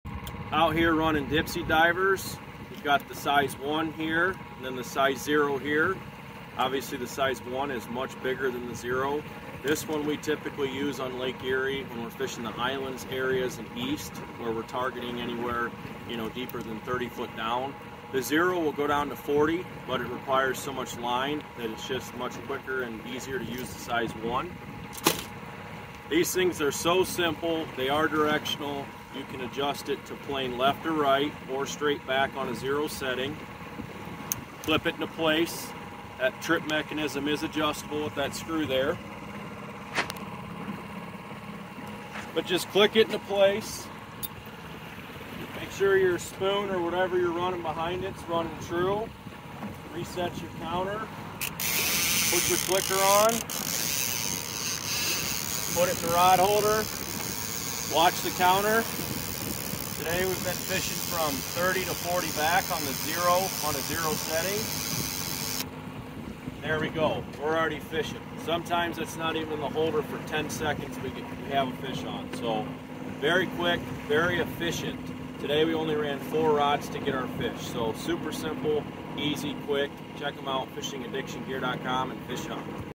Dipsy Diver Walleye - Lake Erie Walleye Trolling
Trolling Dipsy Divers for Lake Erie Walleye. A very fast, effective way to fill the cooler with walleye. Once the water reaches the mid 50's, this method is hard to beat on most days!